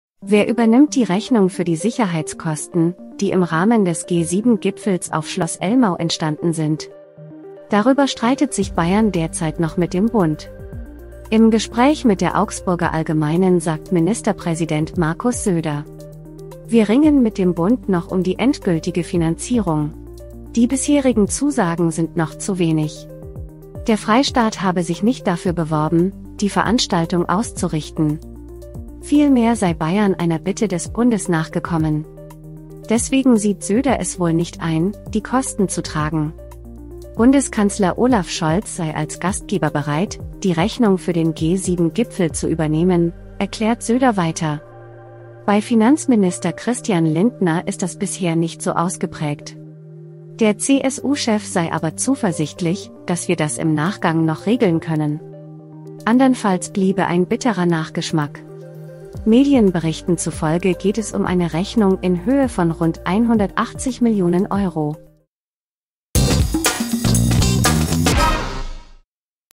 Zoff zwischen Bayern und dem Bund: Wer übernimmt die G7-Kosten?
Der G7-Gipfel auf Schloss Elmau ist vorbei, die Rechnung noch offen. Doch wer übernimmt die Kosten? Darüber streitet der Freistaat Bayern laut Ministerpräsident Söder mit dem Bund.